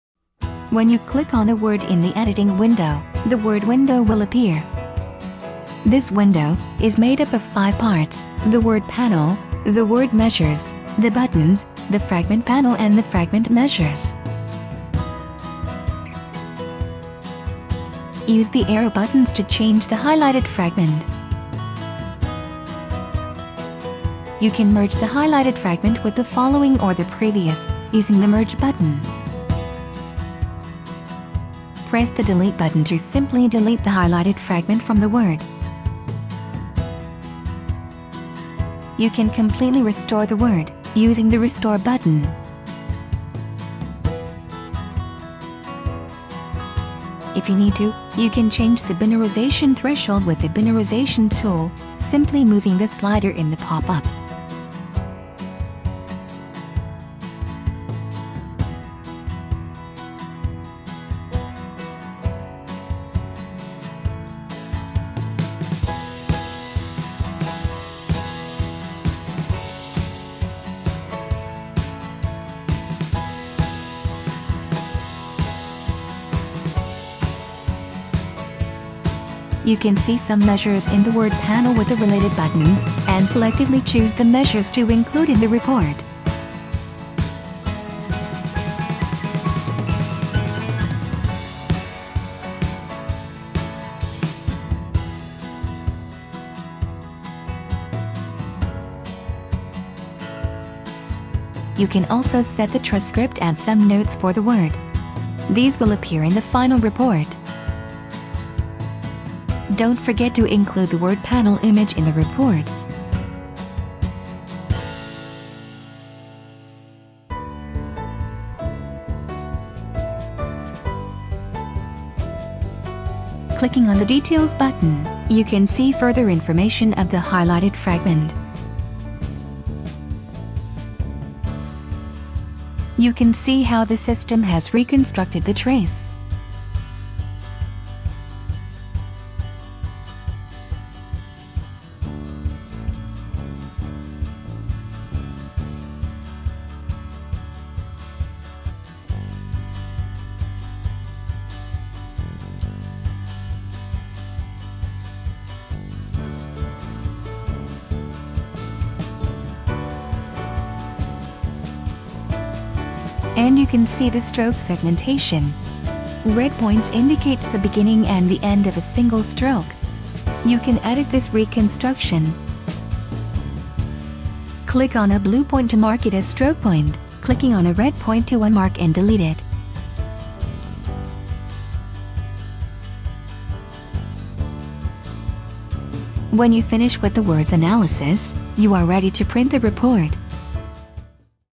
Masquerade® (NEW VERSION) ● (Tutorial #5) Word Analysis
Masquerade® is a software powered by NITe that measures the morphological features commonly investigated by the forensic experts for carrying out the handwriting analysis of questioned documents.

Masquerade® è un software realizzato da NITe che permette di misurare le caratteristiche morfologiche comunemente utilizzate dai periti grafici giudiziari nell'analisi dei documenti manoscritti.